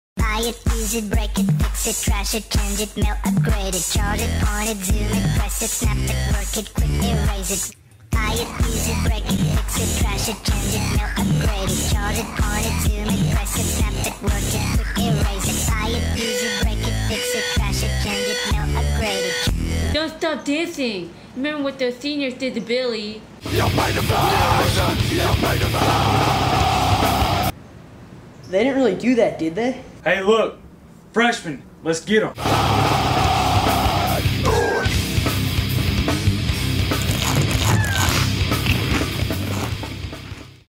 "Freshmen With iPods" by William Noah Schnapp
"Freshmen With iPods" was made by William Noah Schnapp at Episcopal High School for ETv Episode I in October 2006.